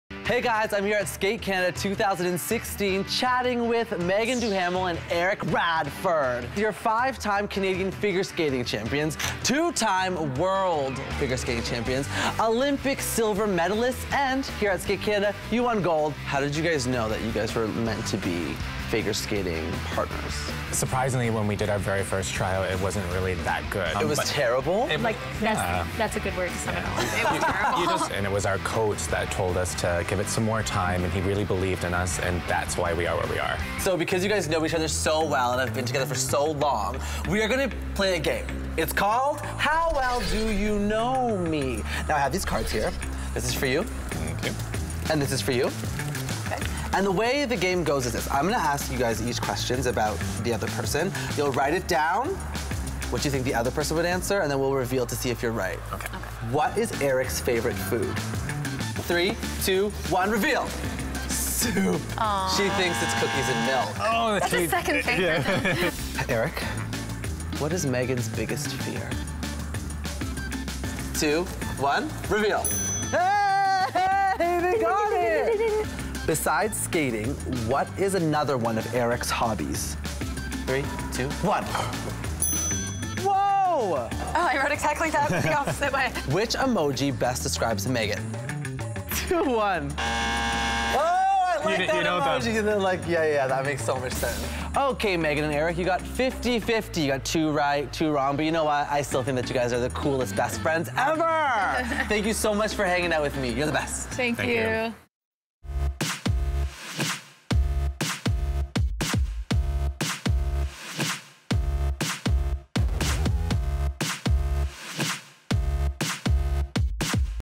Eric Radford & Meagan Duhamel: How Well Do You Know Me???
Suki plays a round of How Well Do You Know Me with Meagan Duhamel and Eric Radford at Skate Canada.